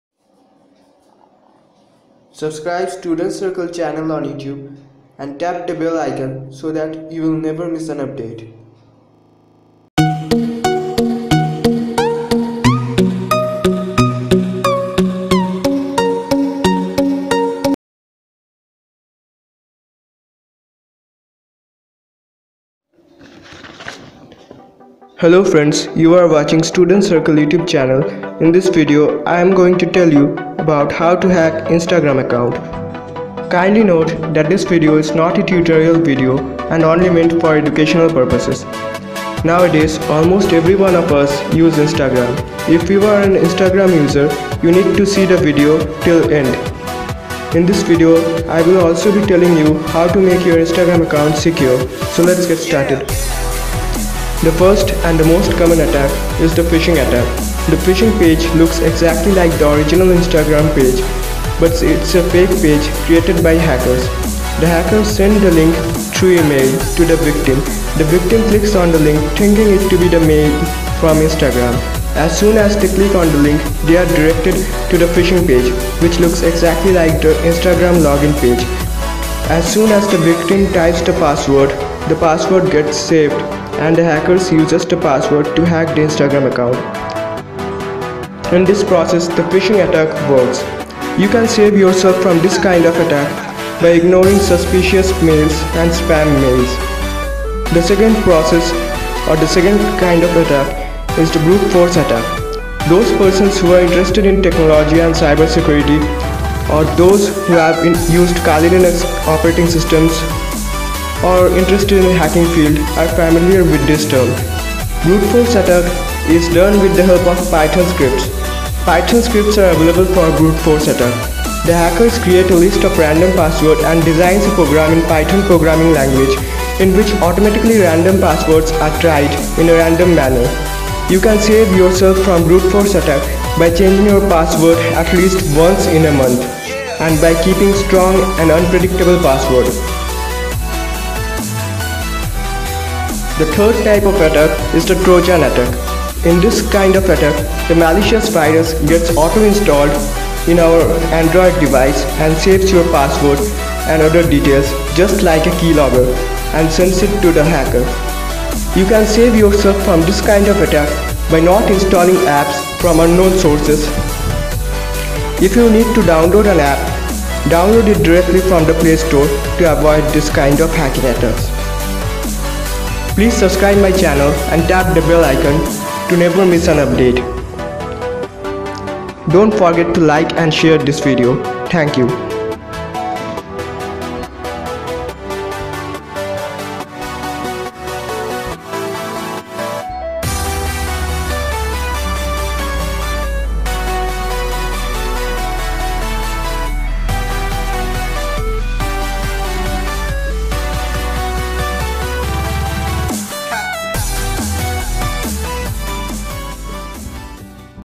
Instagram Users Must Watch| Be safe from Hackers
hello friends, you are watching students circle youtube channel, In this video i'm going to tell you how to hack instagram account, kindly note thet this video is not a tutorial video & only meant for educational purposes. Nowadays almost everyone of us use instagram, if you are an instagram user you need to see the video till end, in this video i'll also tell you how to make your intstagram secure, so lets get started. The first & the most common attack is the phishing attack, the phishing page looks eaxactly like the original Instagram page, but its a fake page created by hackers, the hackers sent the link through email to the victim, the victim clicks on the link thinking it to be the mail from instagram, as soon as they click on the link they are directed to the phishing page, which looks exactly like the Instagram login page, as soon as the victim types the password the password gets saved & the hackers uses the password to hack the Instagram account.
You can save yourself from this kind of attack by ignoring suspicious & spam mails. The second process is Bruteforce attack, those persons who are interested in Technology & cybersecurity or rthose who have used Kali Linux Operating system are familiar with this term. Bruteforce attack ius done with the help of Python scripts, the hackers create a lists of random passwords & designs a program in python programming language in which automatically random passwords are tried in a random manner. You can save yourself from brute force attack by changing your password atleast once in a month and by keeping strong & unpredictaable password.
at third comes the trojan attack, in this kind of attack the malicious virus gets auto installed in your android device & saves your passwords & other details just like a keylogger & sends it to the hacker. You can save yourself from this kind of attack by not installing apps from unknown sources. If you need to download an app download it directly from the play store to avoid this kind of hacking attacks.